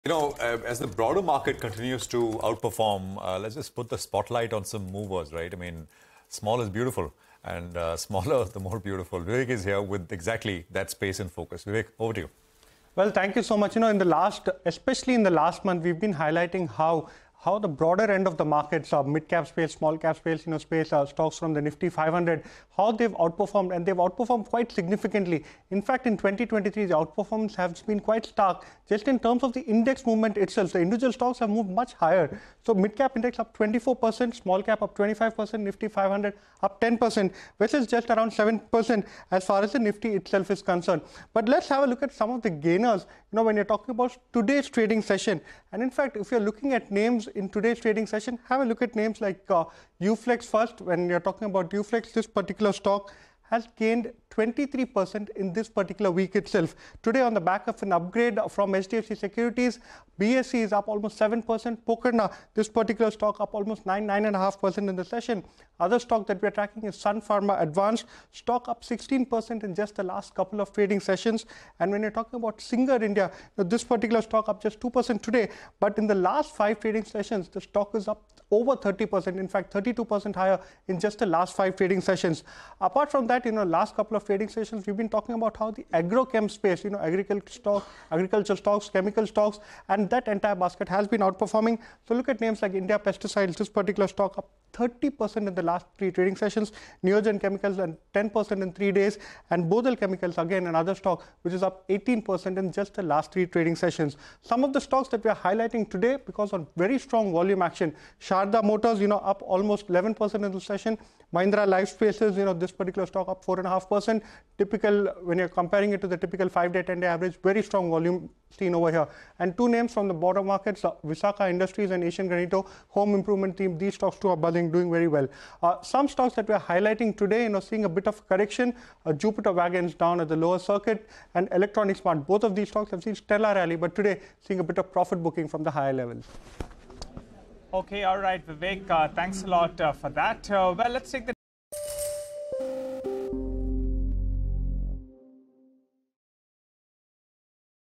Nifty Midcap & Small Cap Have Raced Past The Frontline Indices To Record Highs | CNBC TV18
Broader Markets Outperform | Nifty Midcap and SmallCap have raced past the frontline indices this year so far. Vivek Iyer analyses their performance and lists the big movers.

n18oc_business